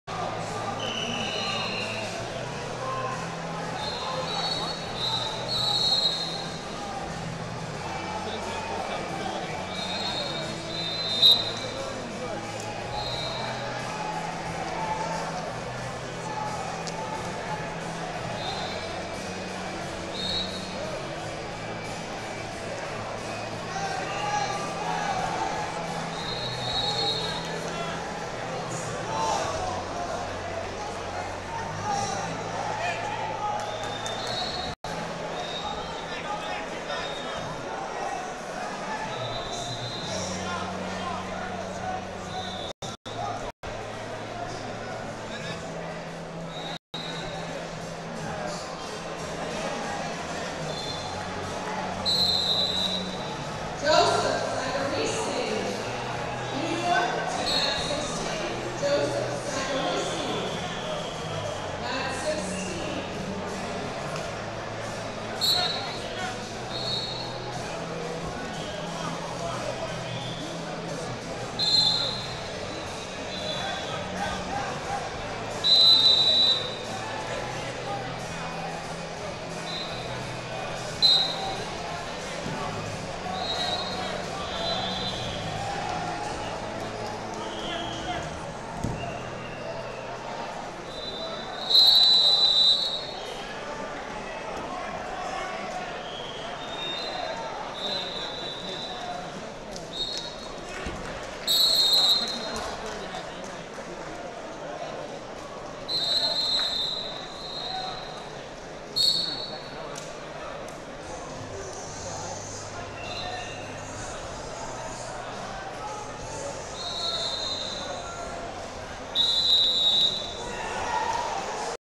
138 Kg Round Of 32 - Giovanni Scafidi, Invictus Wrestling Club Vs Tyler Roe, Pennsylvania 72c1
FloSports/2022 Phil Portuese Northeast Regional Championships - 138 Kg Round Of 32 - Giovanni Scafidi, Invictus Wrestling Club Vs Tyler Roe, Pennsylvania 72c1